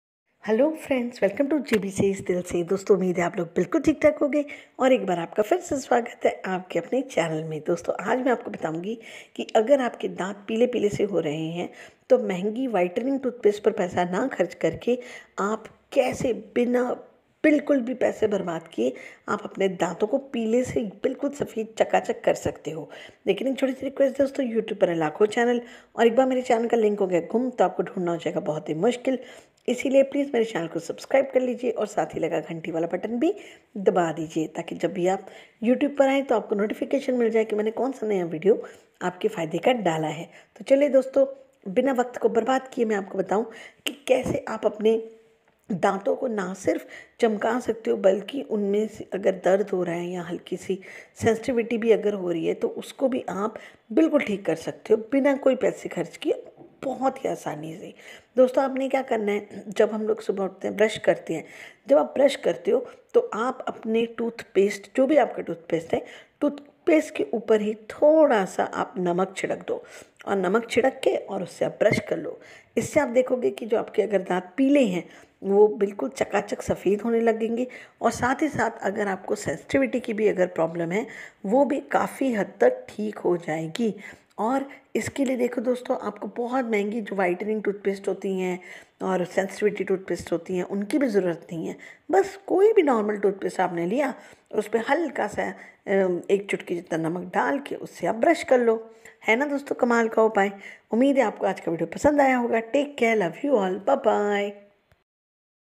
Ghar pe Banaye Whitening Sensitive Toothpaste/ Turn Normal Toothpaste into Whitening Sensitive one
Hello Friends,
Ghar pe hee baniye mehengi,sensitive toothpaste wo bhi seconds me.Janane ke liye dekhe ye video